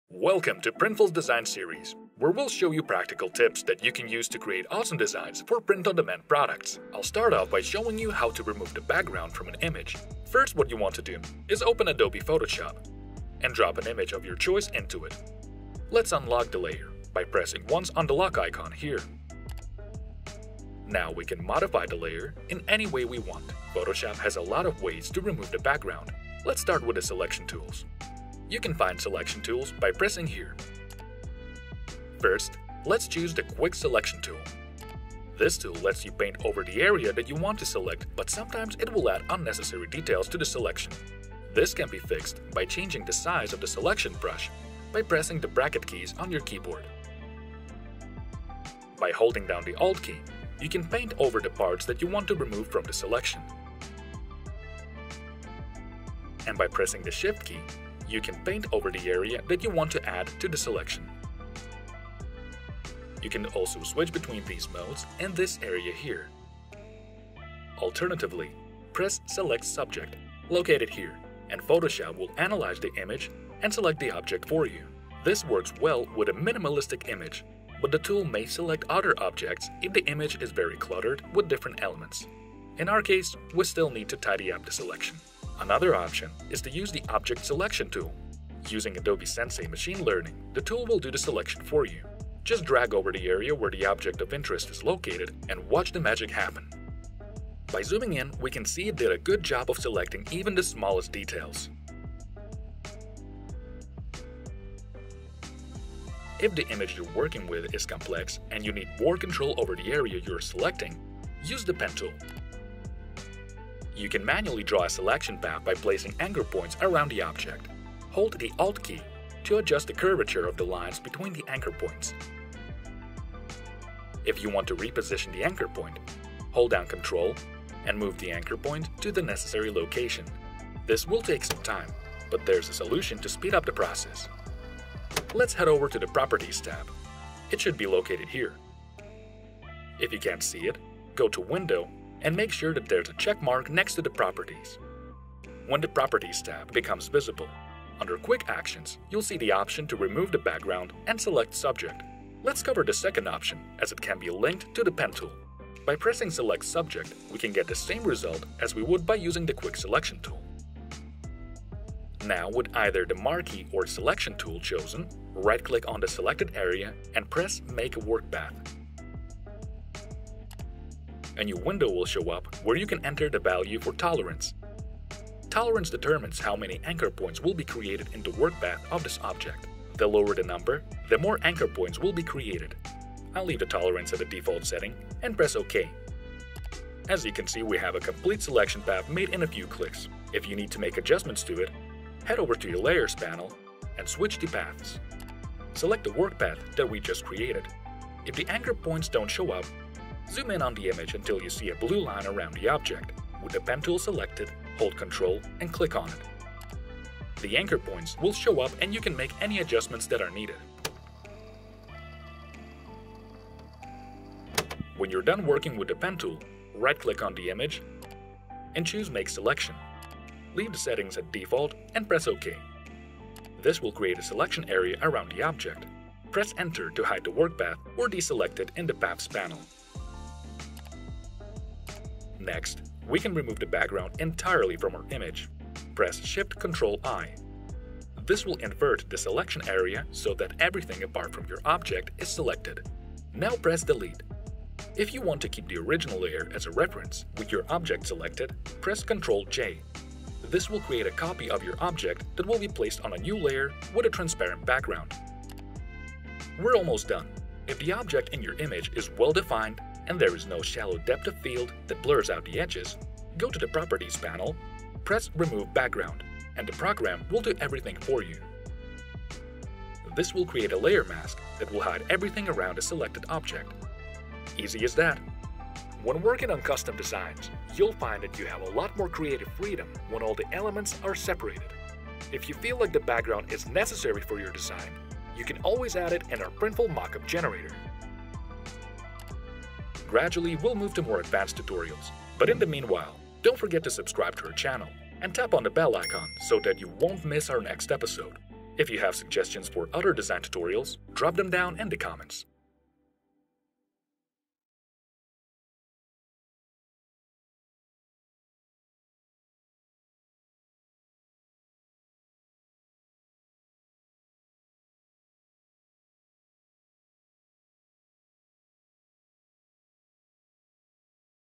How to Remove Background From an Image in Adobe Photoshop: Printful Design Series
Do you want to create awesome designs for print-demand-products, and learn useful tips along the way? Then check our new Printful Design Series.

In this episode, we cover several ways to remove the background from an image! Choose the method you like the best and start creating!

Contents:

00:00:00 - How to Remove a Background From an Image
00:00:31 - Removing the Background With the Quick Selection Tool
00:01:25 - Removing the Background With the Object Selection Tool
00:01:50 - Removing the Background With the Pen Tool
00:02:12 - Removing the Background With Quick Actions

Monday to Friday from 9am to 8pm EST